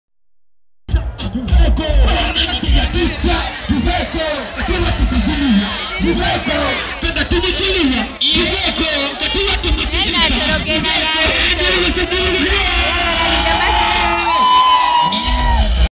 Unforgettable Moments: Highlights and Performances from 'The Best of Sajja Lyayesu' Concert
Relive the magic of "The Best of Sajja Lyayesu" concert at Alinor Gardens Nansana on March 03, 2024, with this unforgettable highlights and performances video! Join us as we revisit the evening filled with music, joy, and celebration, showcasing the best moments from this incredible event.